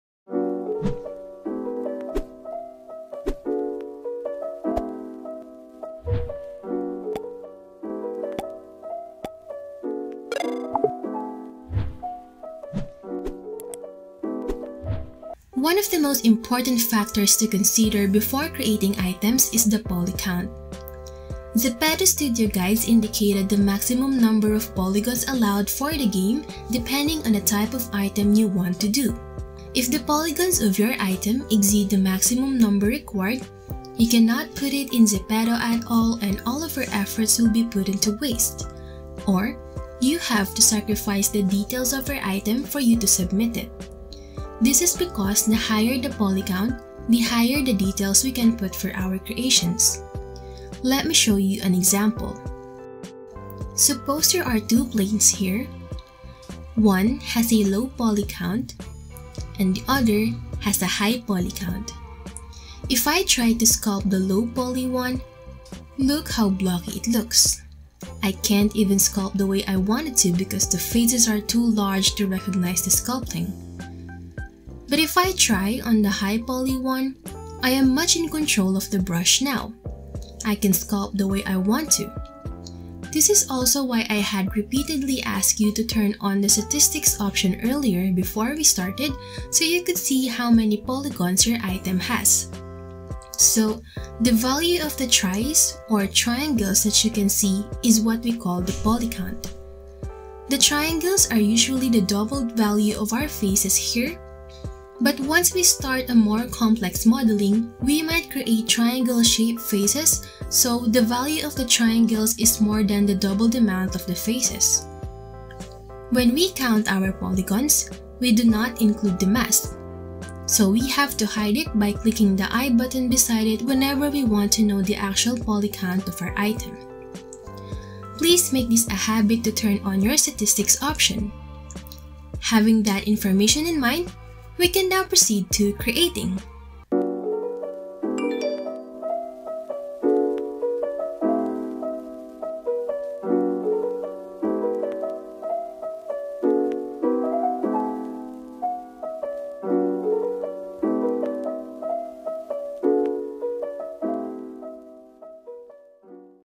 [ZEPETOR CLASS] ZEPETO 3D Item Tutorial 3: Polycount - What is it and Why it Matters
Hello everyone! Item Mentor Maria here! This short video talks about one of the most important factors to consider before creating an item for ZEPETO. Understand what is poly and why it matters in a game.

These videos were accessed by Mentor Maria's mentees 1 week before posting in YouTube.